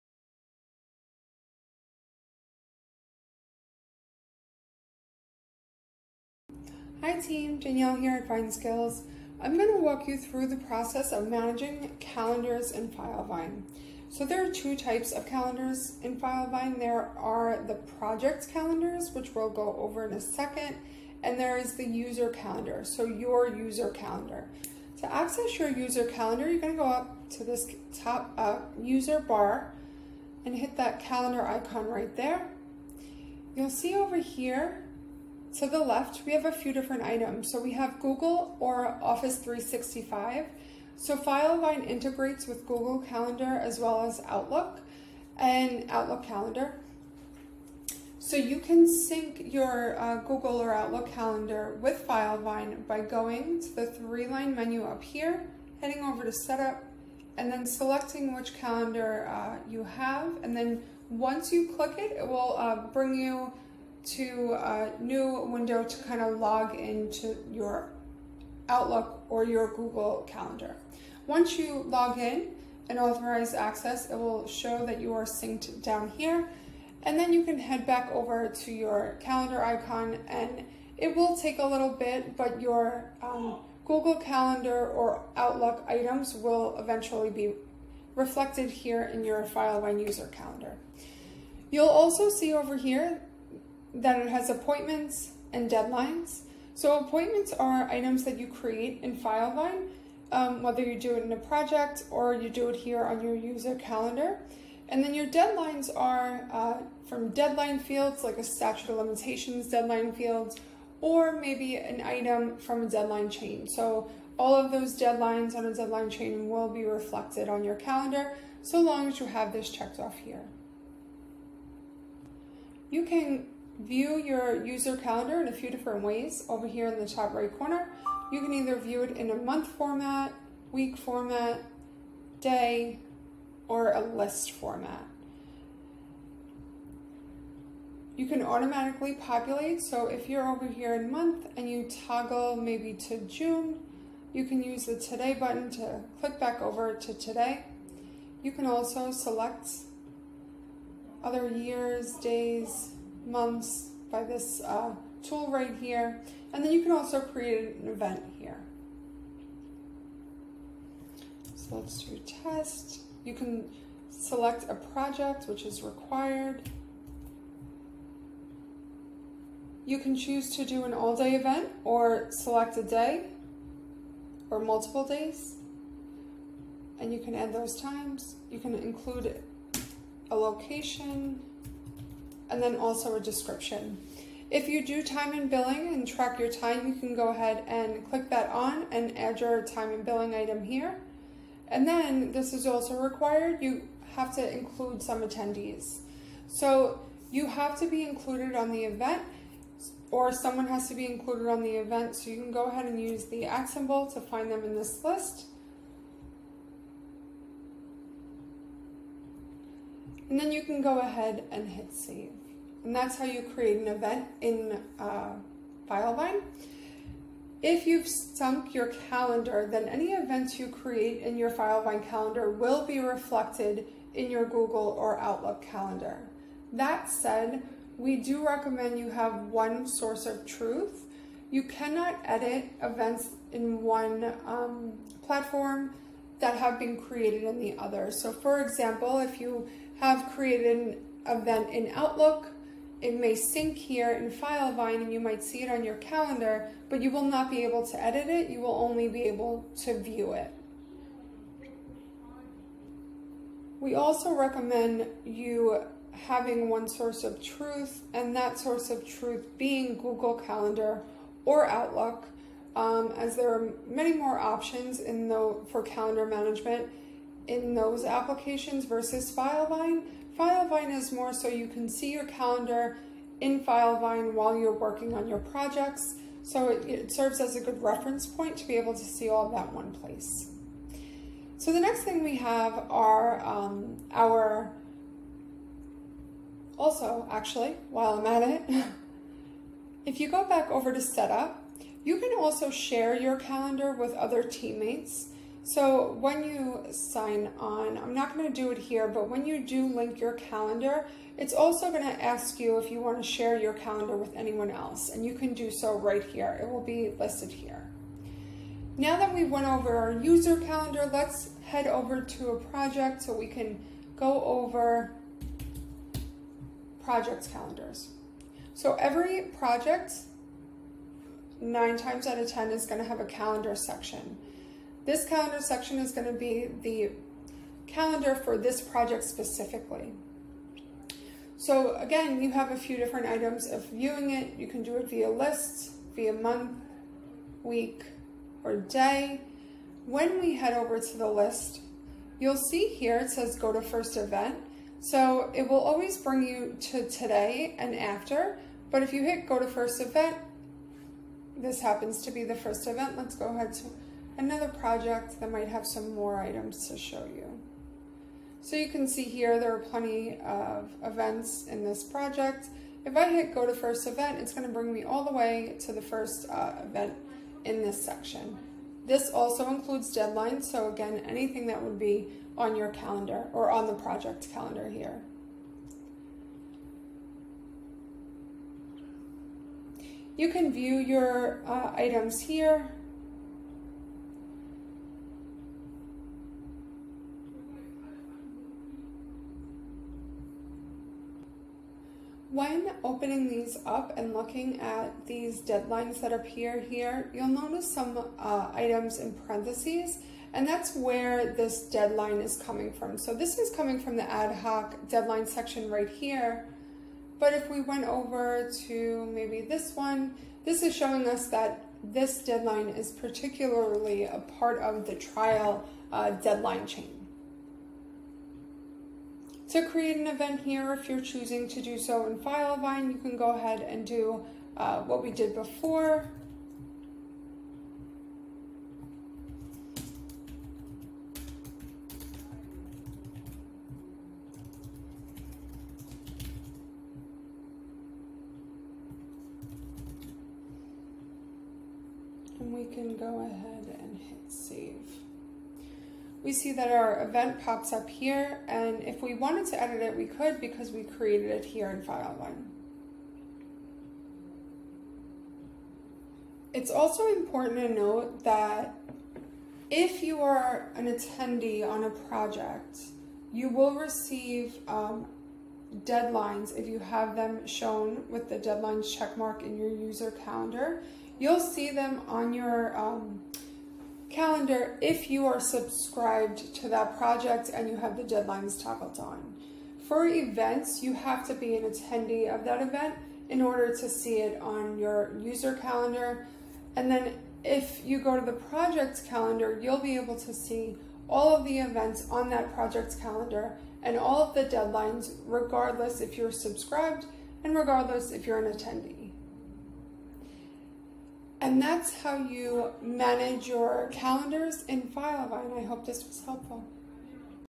Managing Calendars in Filevine
Optimizer Danielle goes through the basics of managing calendars in Filevine, including both project and individual user calendars (from how to sync your Google or Outlook calendar with the one in Filevine, to editing calendar events, and more).

Get help using Filevine, from customizations to training and beyond, with the expert team at Vineskills